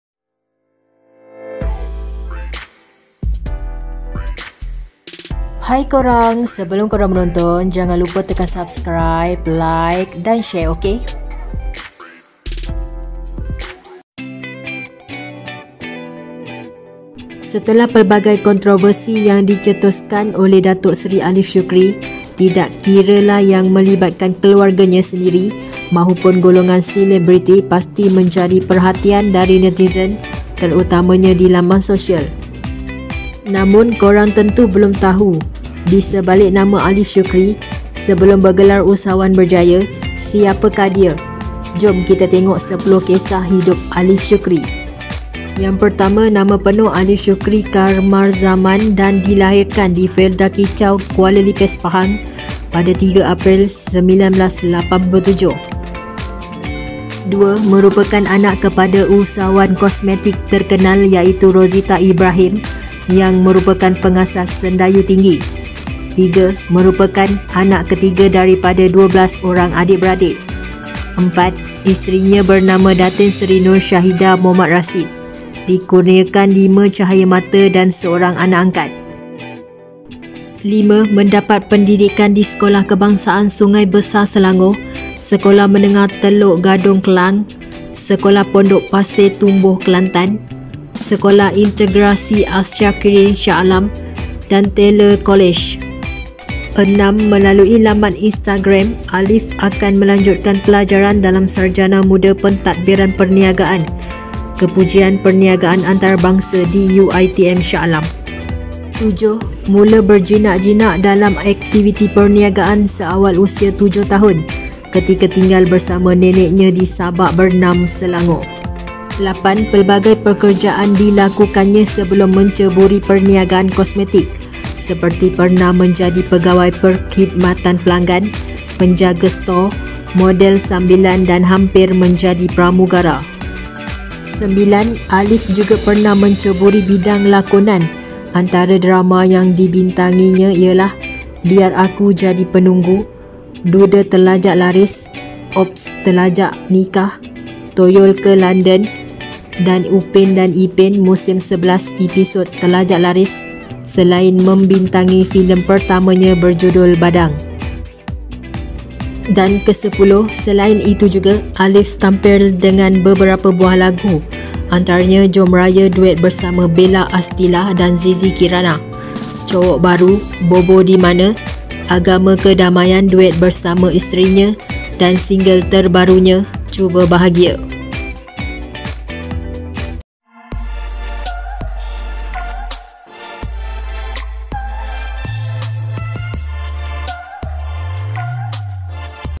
10 Kisah Datuk Seri Aliff Syukri Sebelum Bergelar Usahawan Kosmetik
Kisah hidup aliff syukri, dari zaman sekolah sehingga Bergelar Usahawan berjaya. Kejayaan nya bermula dari bawah sehinggalah menjadi seorang yang berjaya.. Nak tahu lebih lanjut, lihatlah video yang telah admin sediakan.

Love from Idrose Channel

Kisah Artis-artis Popular Tanah Air

Sumber : Google, Instagram